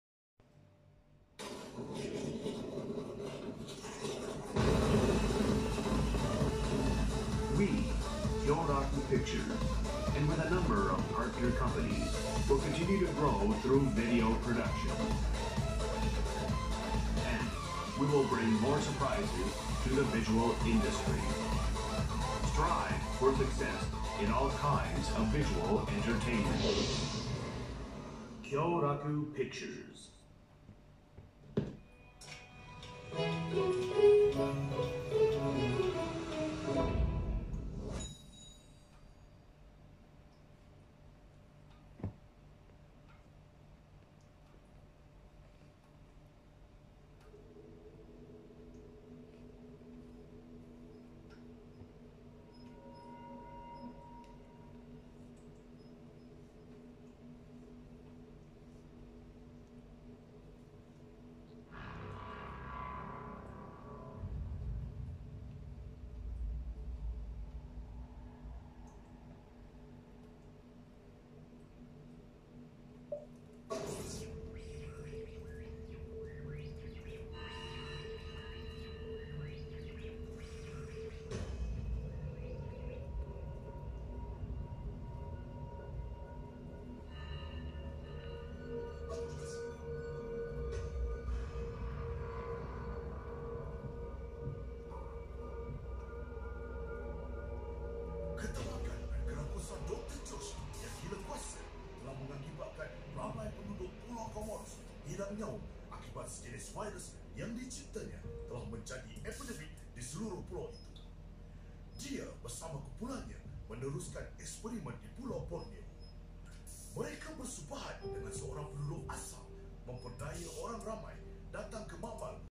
Belaban Hidup in Japan
Belaban Hidup movie screening in biggest Horror film festival in Japan, Yubari Fantastic
Film Festival Hokkaido on 16/9/21👆